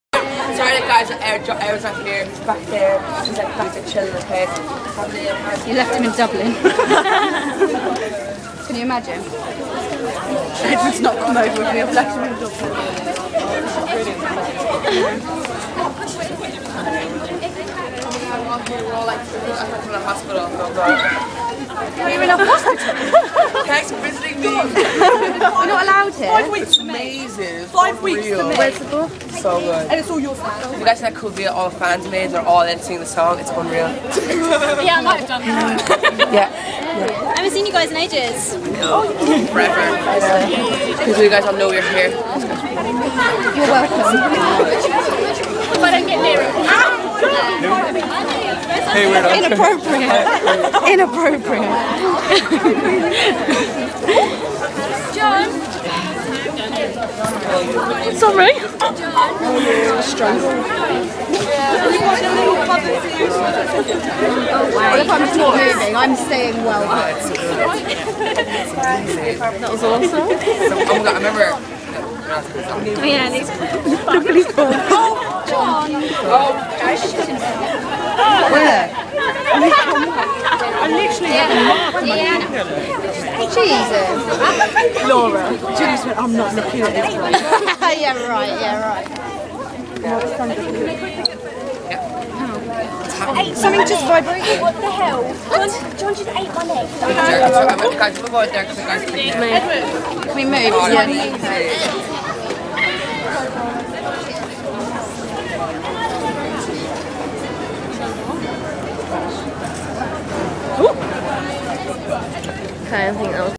john and edward arriving at heathrow 10/05/12 for bgmt
john and edward arriving